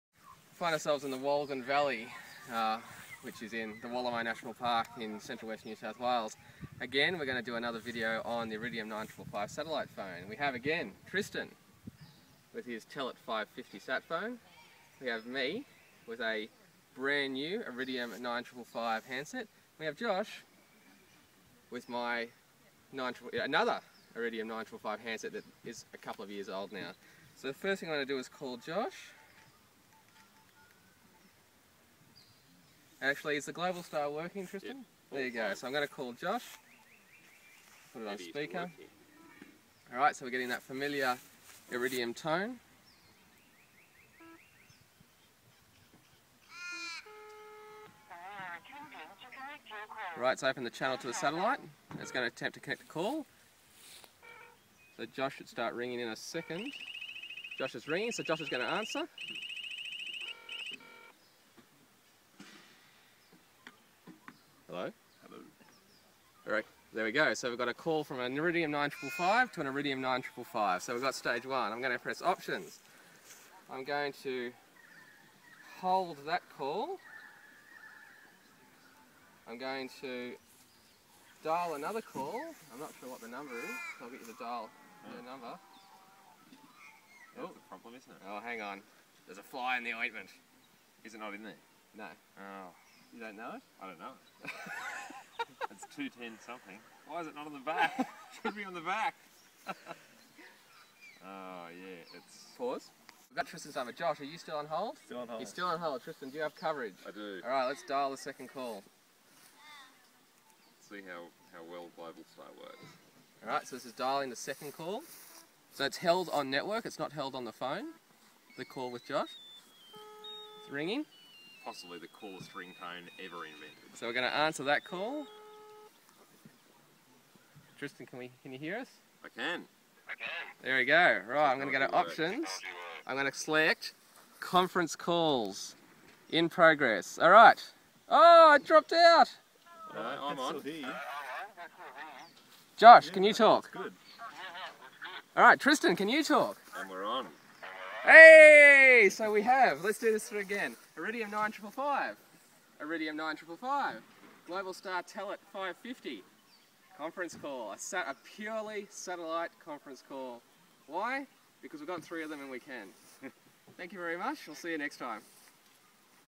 Iridium 9555 x 2 and Telit 550 Conference Call
A purely satellite phone conference call, between two Iridium 9555 phones and a Telit Sat 550 on Globalstar.